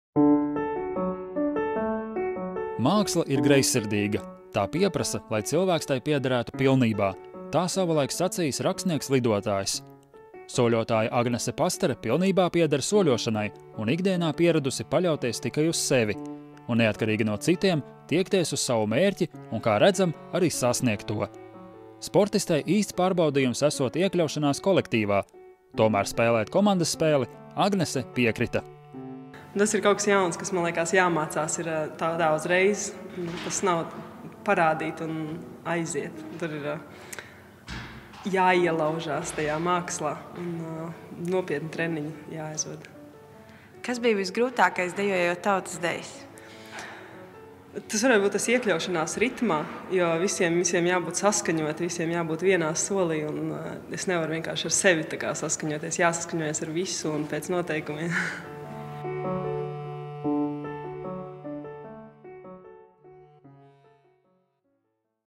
Soļotāja Agnese Pastare – ''Gada sportiste 2016''
Māksla ir greizsirdīga: tā pieprasa, lai cilvēks tai piederētu pilnībā – tā savulaik sacīis rakstnieks Lidotājs. Soļotāja Agnese Pastare pilnībā pieder soļošanai un ikdienā pieradusi paļauties tikai uz sevi. Un neatkarīgi no citiem, tikties uz savu mērķi un kā redzam, arī sasniegt to. Sportistei īsts pārbaudījums esot iekļauties kolektīvā un tomēr spēlēt "komandas spēli" Agnese piekrita!